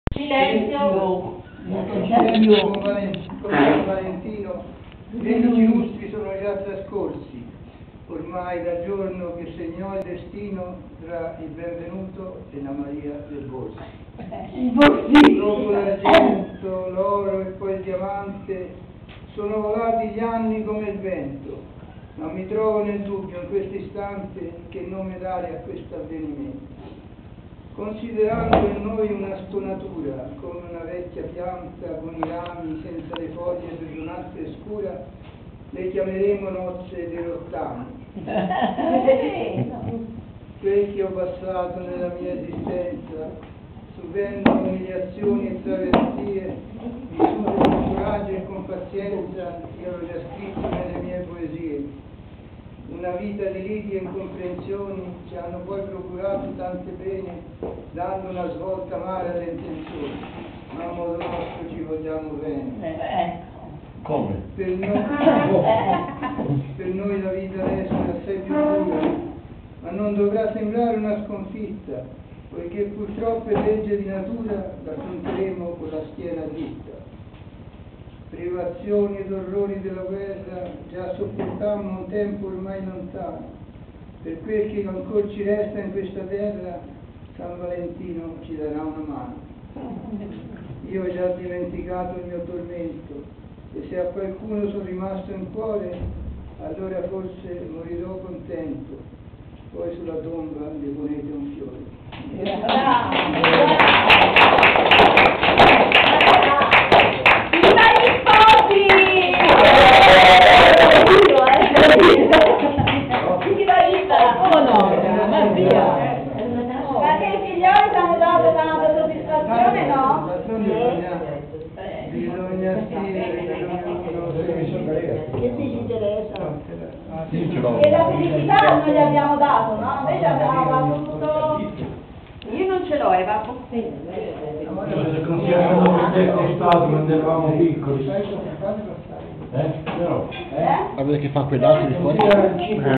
Maria Borsi and Benvenuto Piacentini
Maria Borsi and Benvenuto Piacentini celebrated 65 years of marriage (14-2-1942 to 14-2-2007) in Barga Tuscany Italy